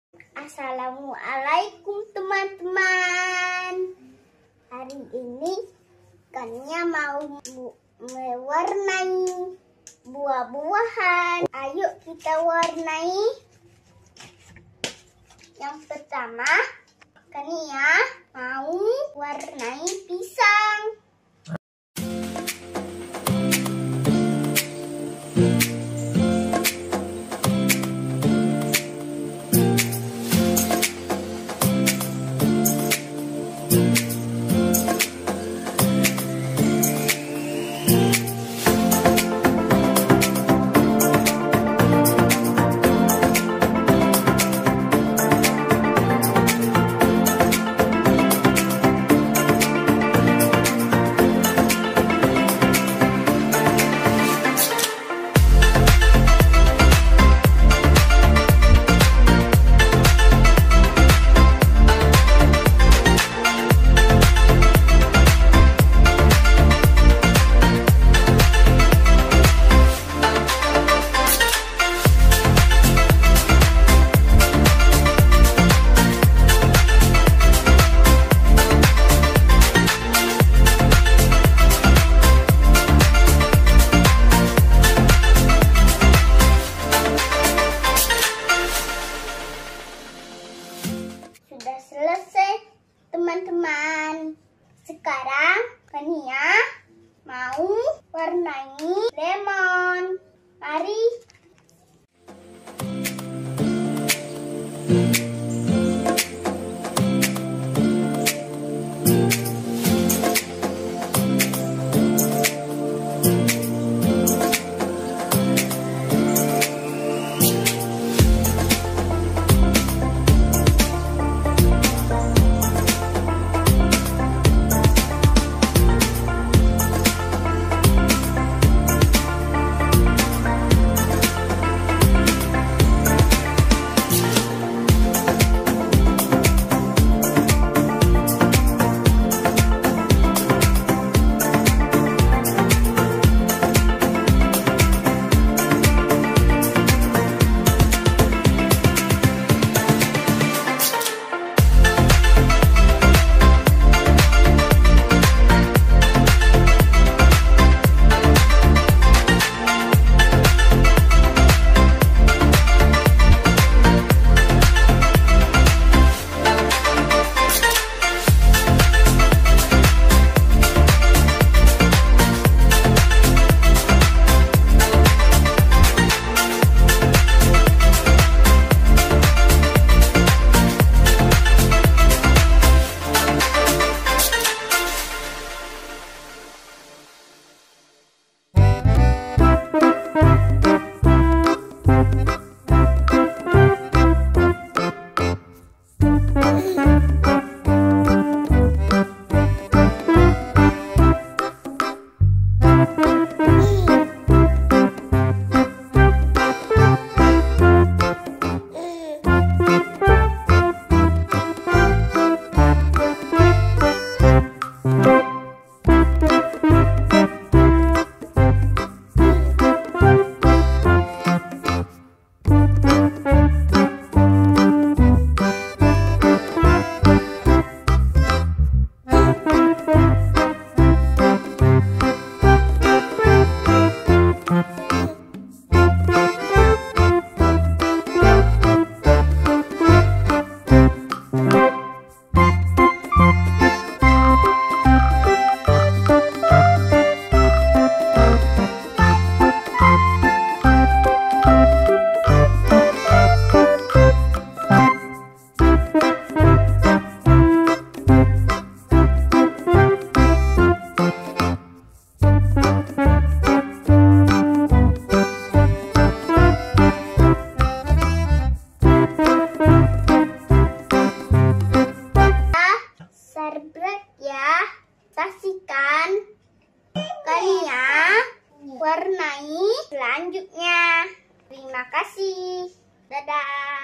Belajar mewarnai gambar lukisan Pisang Lemon | Buah buahan
Belajar mewarnai gambar bisa meningkatkan kecerdasan anak.